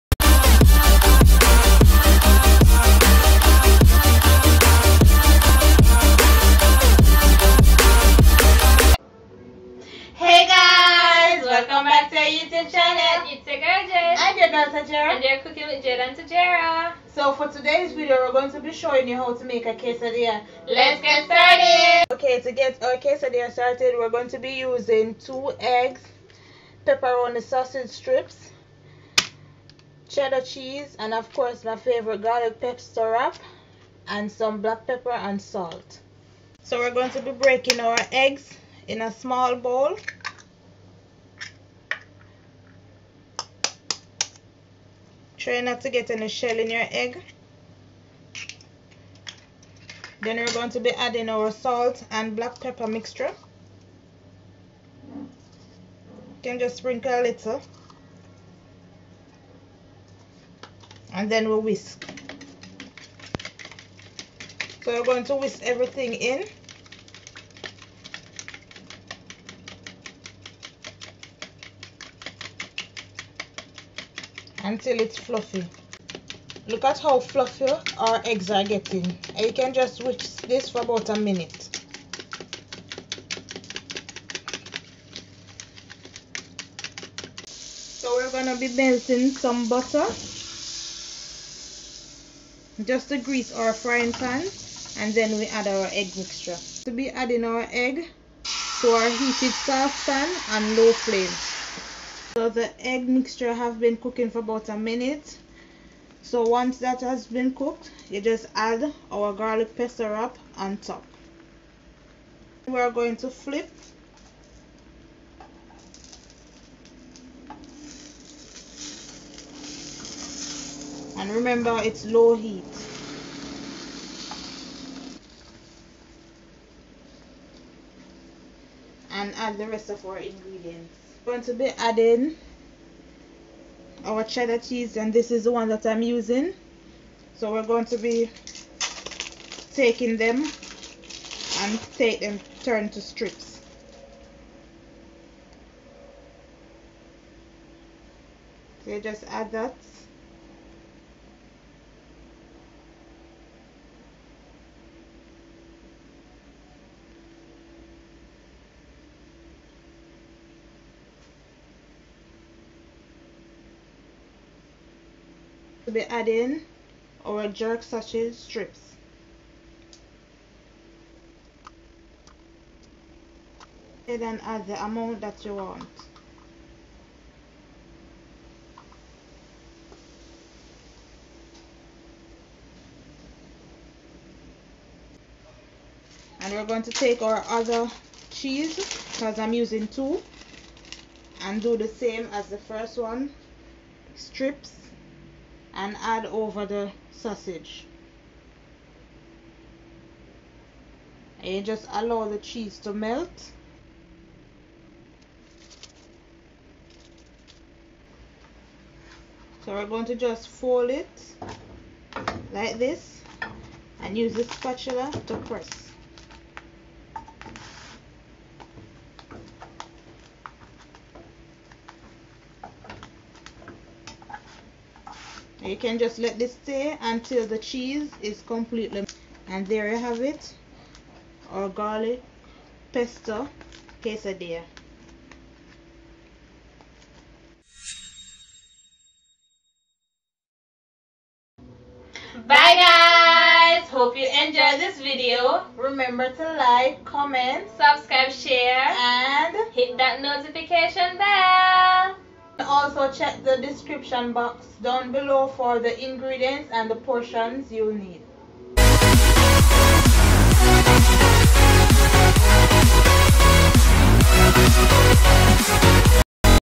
How to make a breakfast Quesadilla
Hey guys,. today we are making breakfast quesadillas!!!

This is an easy egg and cheese quesadilla that can be used as a quick breakfast recipe.  LET'S ENJOY!!

Ingredients you will need:
2 Egg
1 Pepperoni sausage
1 Garlic pesto wrap
1 Cup shredded cheese
Pinch of salt and Black pepper
2 Tbsp butter